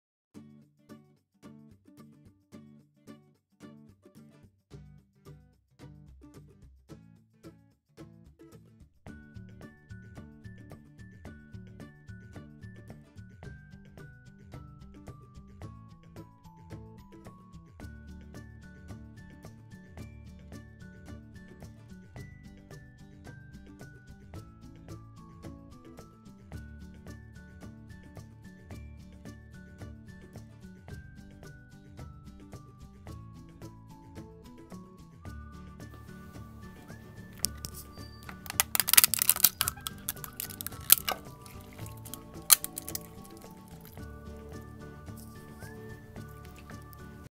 ASMR wax hands making and crash listen to the crash sound
Awesome short video of making and crashing a wax hand , sounds of crashing it is really satisfying!
Enjoy this video and please follow our channel!